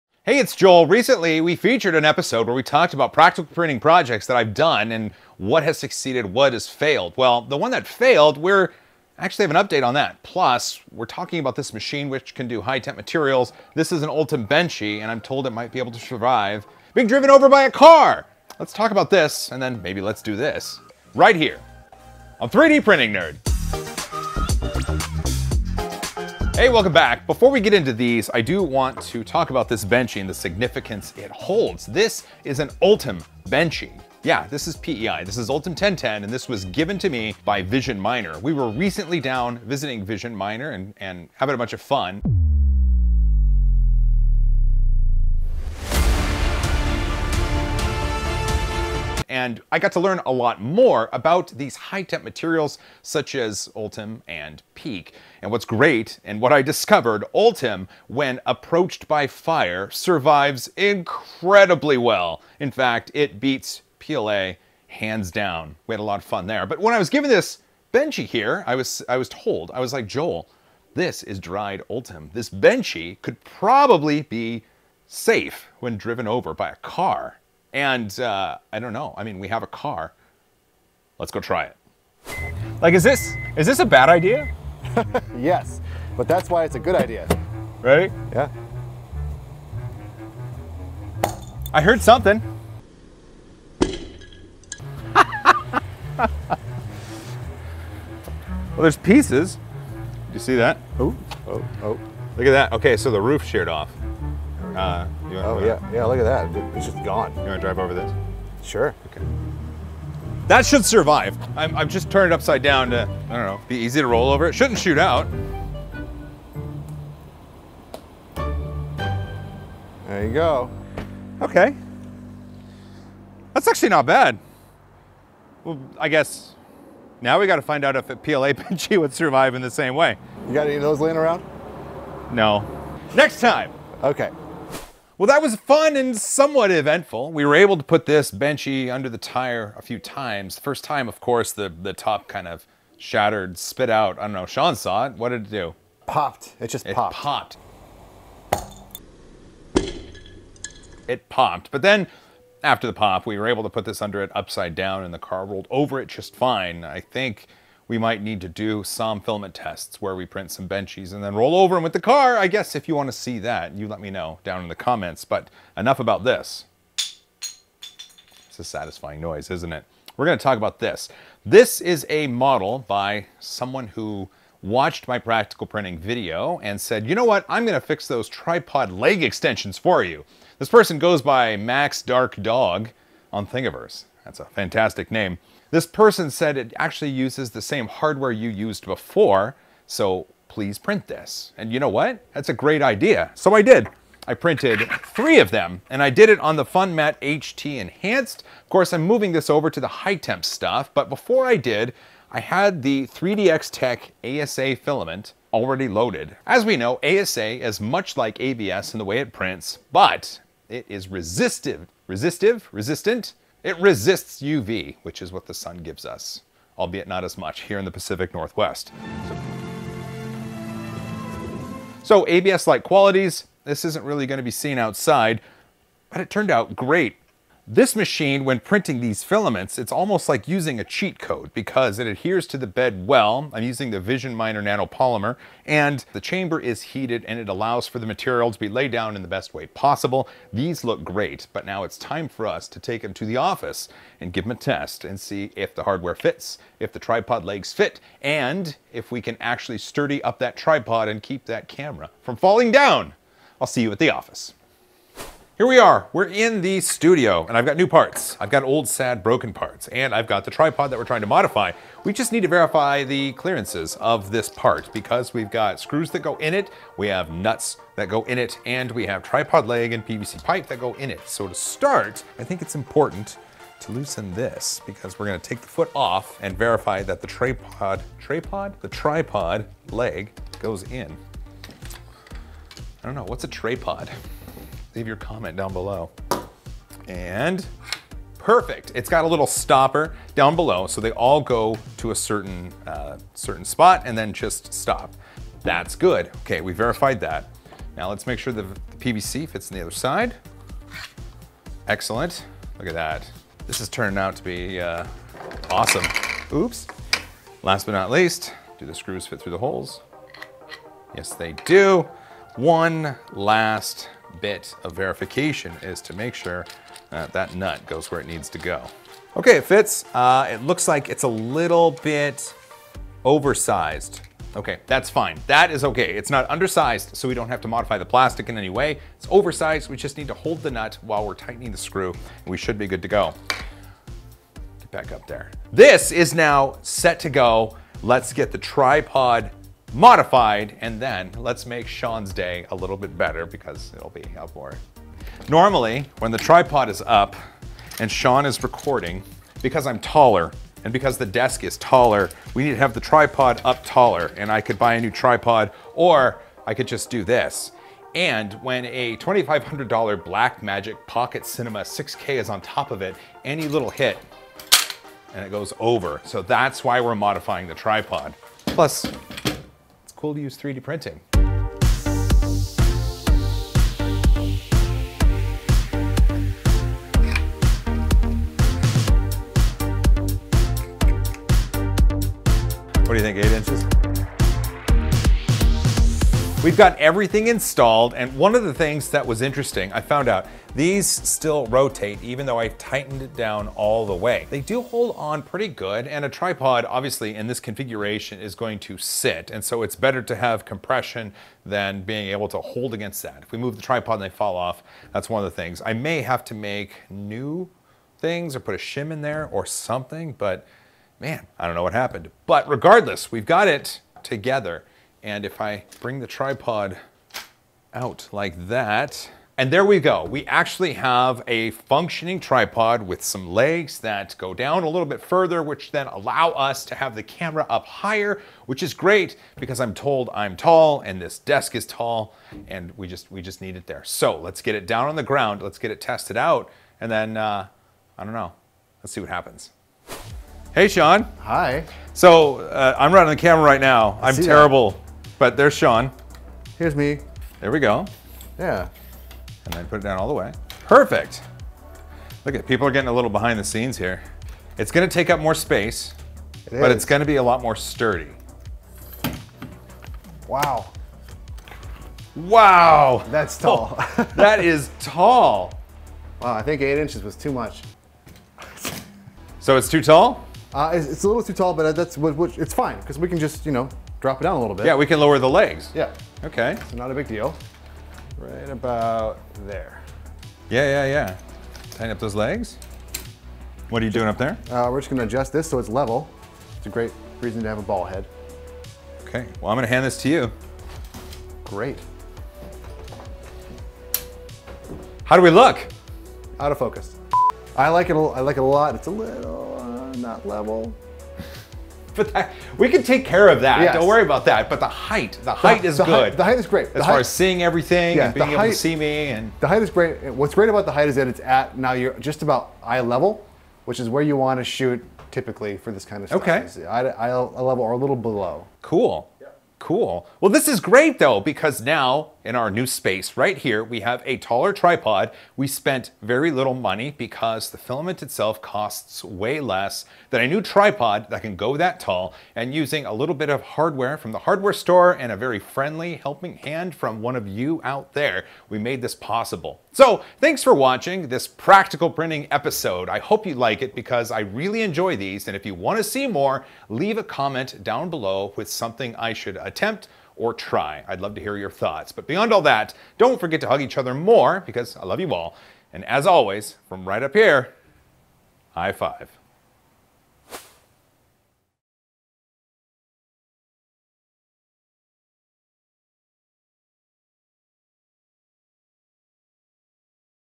Practical Printing Fixes It Again! 3D Printing FTW!
I love practical printing, and 3d printing parts to fix things around the house is AWESOME. My tripod leg extensions never really worked right, and thanks to someone in the community, new ones were printed and WOW DO THEY WORK!!

Find Me Socially!

Want to send me something?
3D Printing Nerd
USA